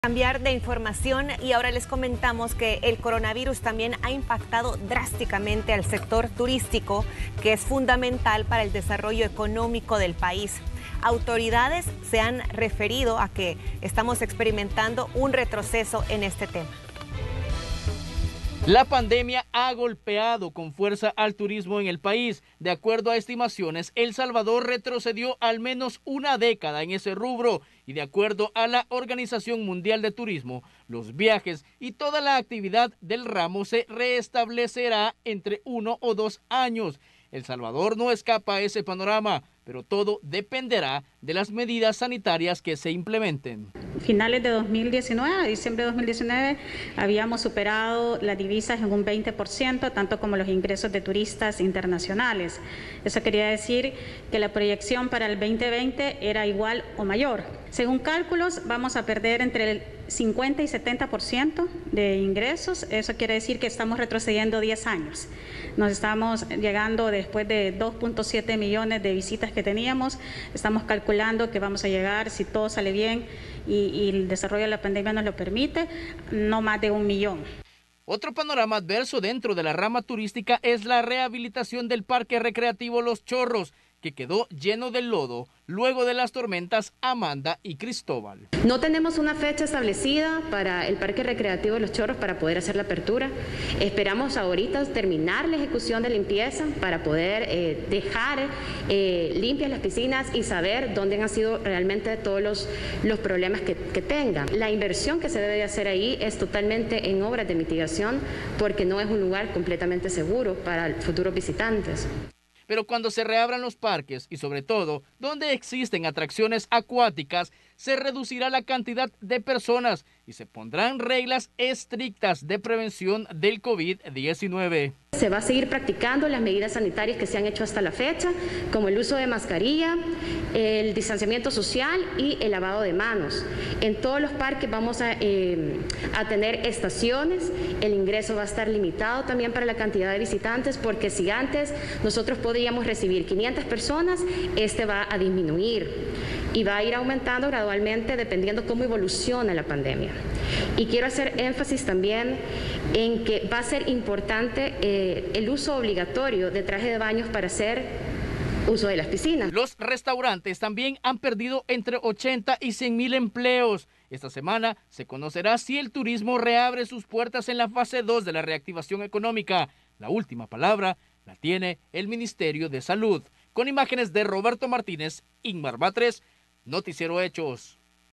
El Salvador retrocede en turismo debido al COVID-19
El coronavirus ha impactado drásticamente al sector turismo, que es fundamental parda el desarrollo económico del país. Autoridades han asegurado que se ha registrado un retroceso en esta materia.